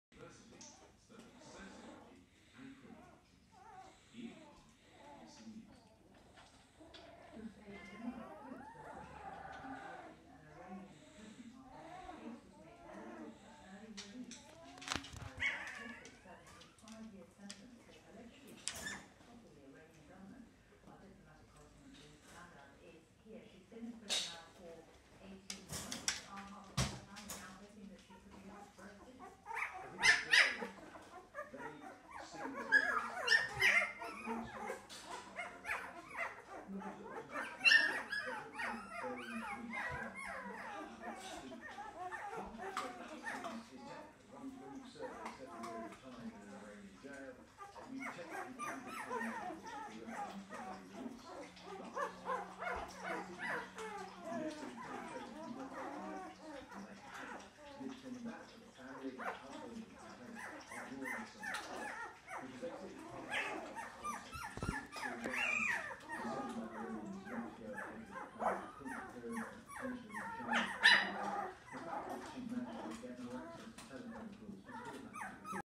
Lady’s Wire Hair Fox Terrier Puppies - 22 days
22 days old up and moving around crying for mum and playing.